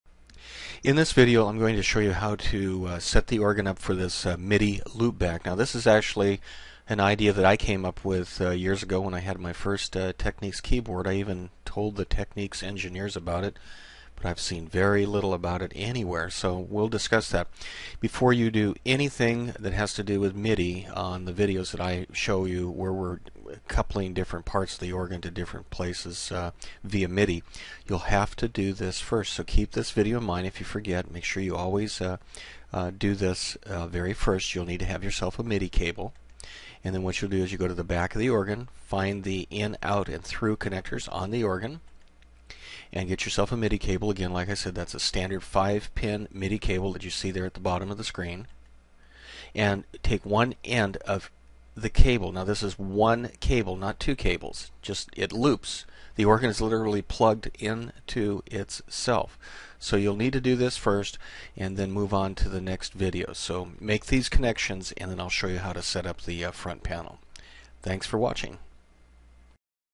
Technics Organ Midi - Loopback Video #1 Connect the Cables
Connect the single midi cable as shown in this video.  The organ is actually sending midi data back into itself thus the word "loop-back" Do this before you proceed to any of the othe midi related videos.  Not pluging the cable in as shown means you will not be connected.   The Technics FA1 is used in this demonstration.  Proceed to video #2 to complete the initial setup.  Other organs can be adapted to perform in a similar manner. Happy experimenting!  Please don't forget to visit my other YouTube Channel 6a65682